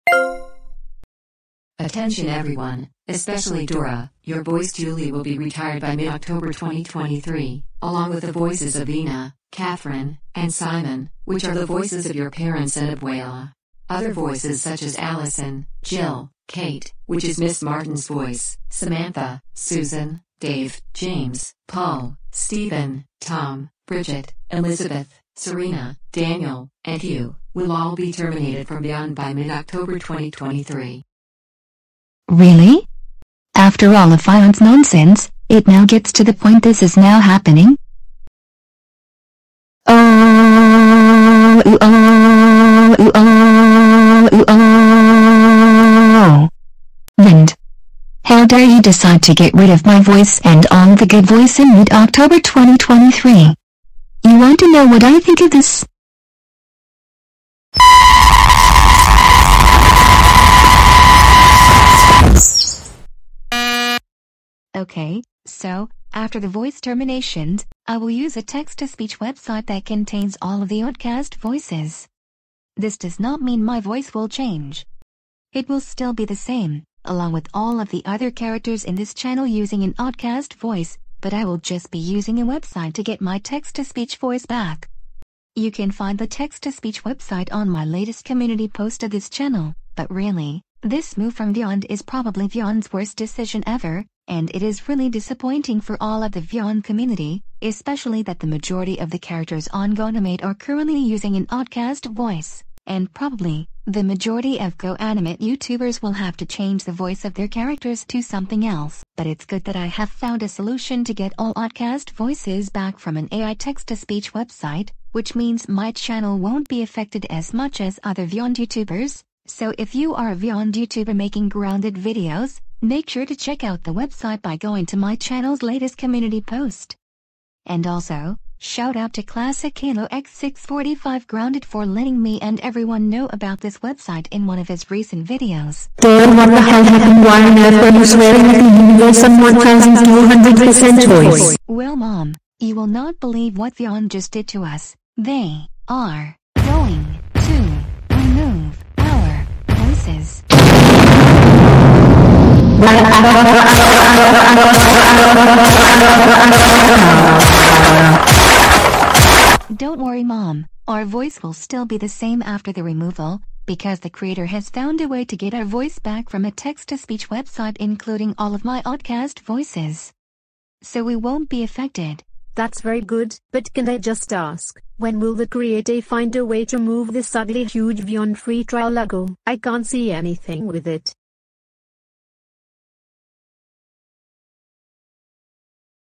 My Response to the Oddcast Voice Removal | Important Channel Updates
This video is going to be about my response about the disappointing move from Vyond to remove all Oddcast voices. I will also be explaining on what the future of our channel will be after the voices have been removed.